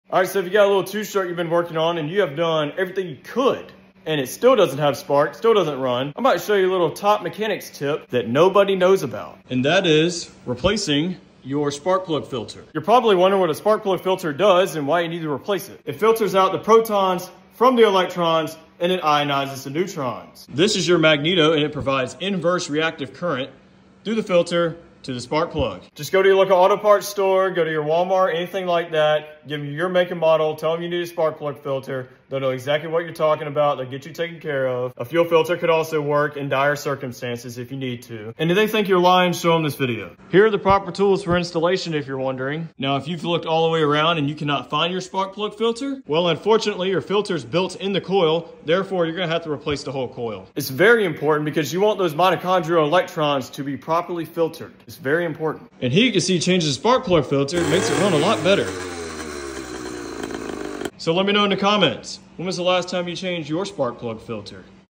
2-Stroke Engine No Spark? Replace the Sparkplug Filter!
If your 2 stroke engine doesn’t have spark, then this video will cover diagnosing and replacing your spark-plug filter!